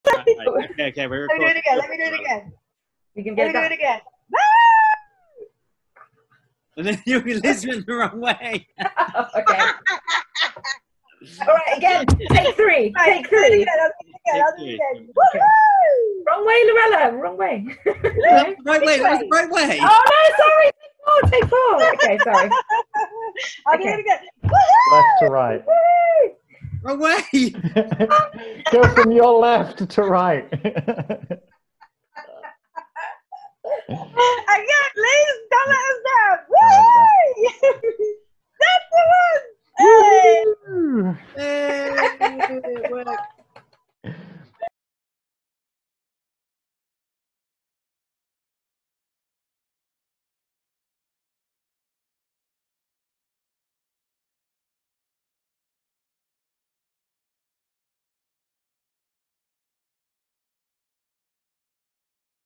Arty-Folks and The Fabularium
Funded by the Arts Council England we are so excited to start our training with The Fabularium Theatre Company who will show us how they run workshops using Zoom and to engage audiences. Joshua and Gareth gave our staff Liz and Lorella a little introduction which was just hilarious! Bless them, they wanted to show us how to do a 'Mexican wave' on Zoom which turned into wild flailing and could be described as an 'English wobble' at best. But see for yourself!